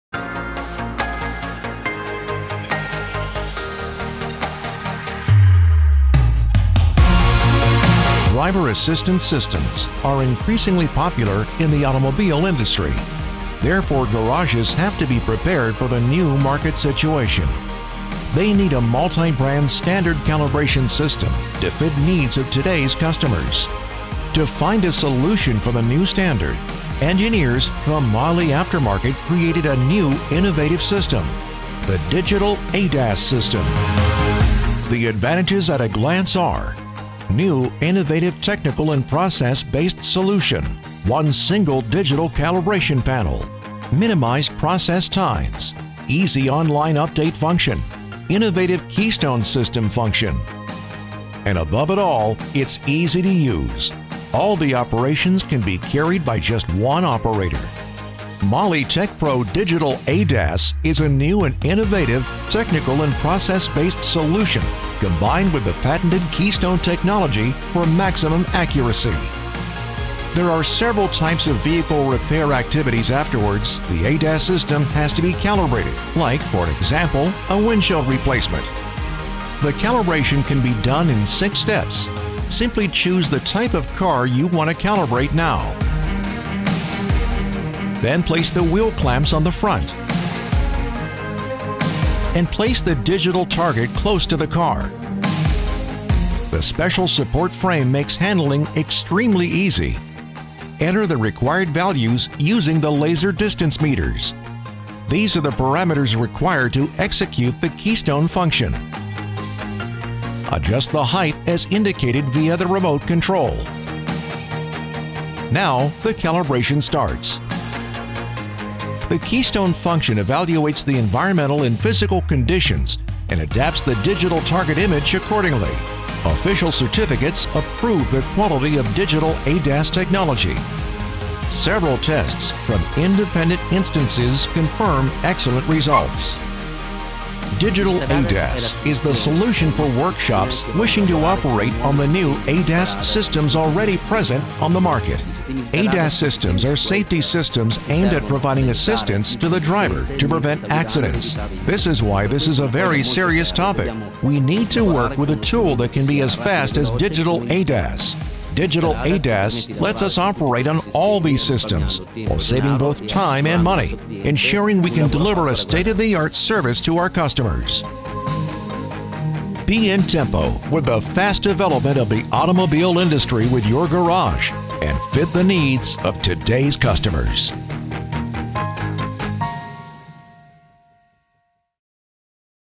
MAHLE TechPRO® Digital ADAS Calibration Tool
The MAHLE TechPRO® Digital ADAS allows workshops to adjust and calibrate driver assistance systems quickly and easily - using just one single digital calibration panel. This saves time and money. The tool is continuously updated online and expanded to cover more vehicles. With this intuitive and sustainable solution, MAHLE is setting new benchmarks.

▬ Favorite Videos ▬▬▬▬▬▬▬▬▬▬▬▬